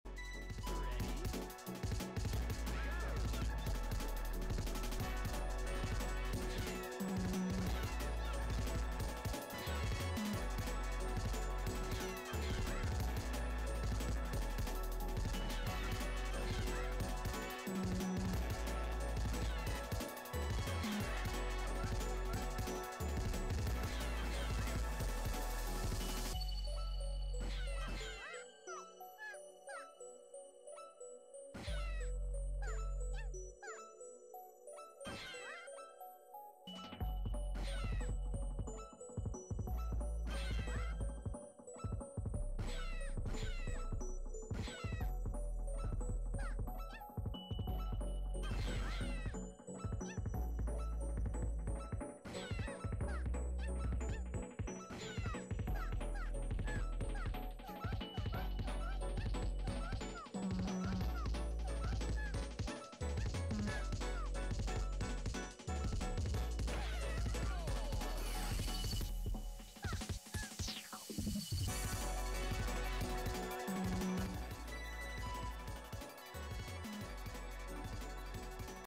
Stardust - Impact Stunt Goal 127,202
Pretty fun, you get lots of attempts to the ending since it's all quite consistent until then. Ending is kinda gambly tho. definitely easier than the scaffolding blue score imo

This strat can get 237-238, even a 239 if you gamble super hard for an edge clip off the star but that would be a questionable life choice. you could maybe speed it up and catch an earlier star cycle for like a 24x (you definitely can for the blue goal) idk I labbed it for maybe 5 minutes go do it yourself if you want to look into it more